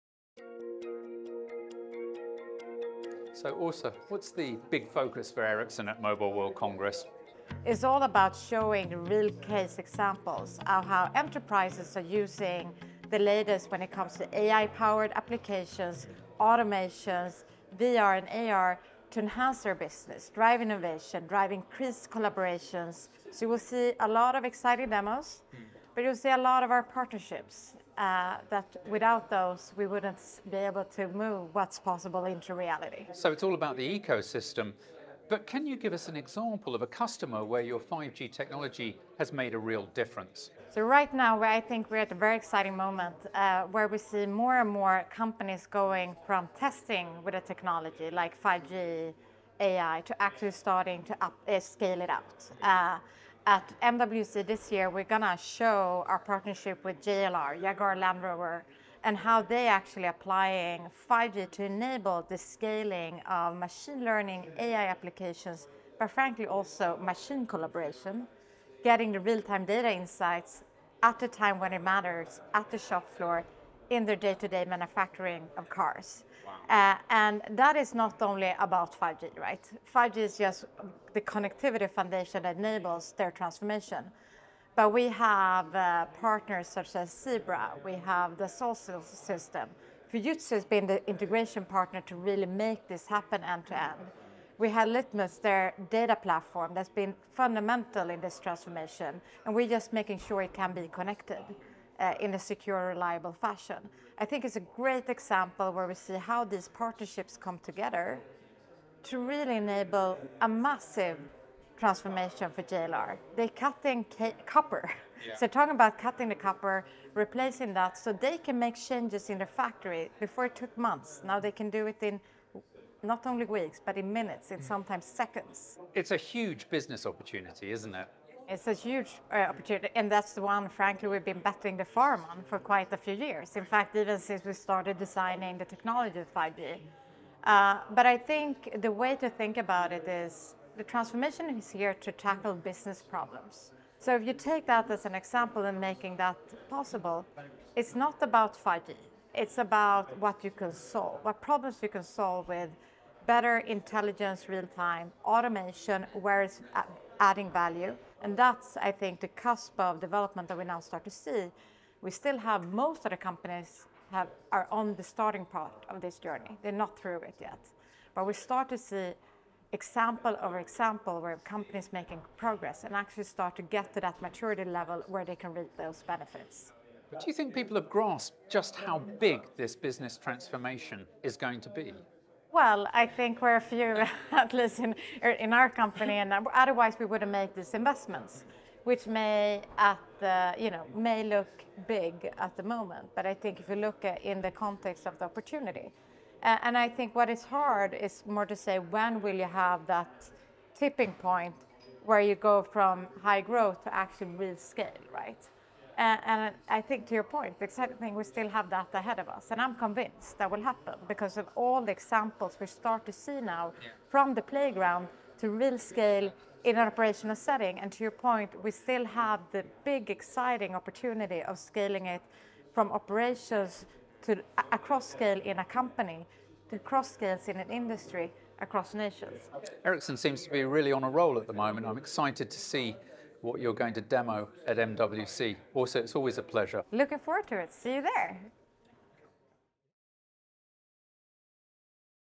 Ericsson showcases AI & 5G-powered business transformation at MWC 2025
How is 5G transforming industries? At MWC, Ericsson showcases how AI, automation, and 5G are driving real business impact. From Jaguar Land Rover’s smart manufacturing revolution to ecosystem-wide innovation—this is the future of enterprise.